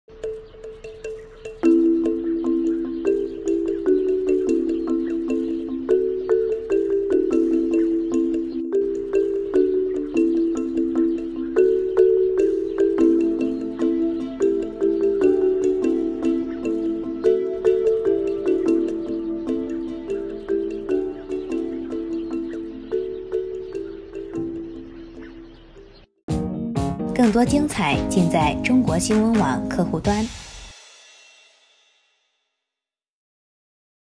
青海玉树出现幻日现象 “三个太阳”同空
在三江源头的青海玉树，2月9日出现“三个太阳”的幻日现象。所谓幻日是大气的一种光学现象。在天空出现的半透明薄云里面，有许多飘浮在空中的六角形柱状的冰晶体，偶尔它们会整整齐齐地垂直排列在空中。当太阳光射在这一根根六角形冰柱上，就会发生非常规律的折射现象。据了解，幻日现象，十分罕见，需要满足的气象条件十分苛刻，并且持续时间很短。